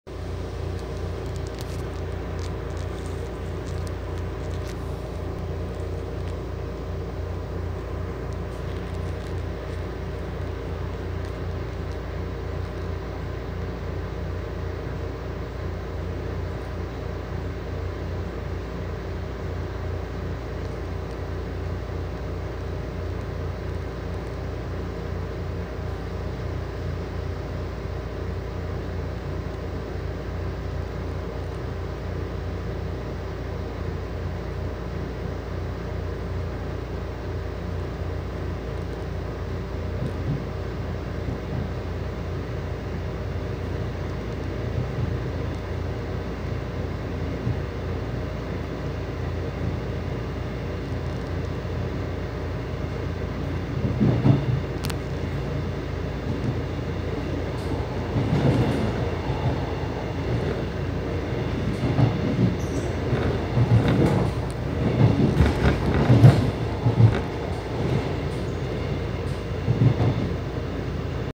[후부 촬영] 용산발 익산행 무궁화호 예산역 발차
[후부 촬영] 용산발 익산행 무궁화호 예산역 발차 영상 입니다.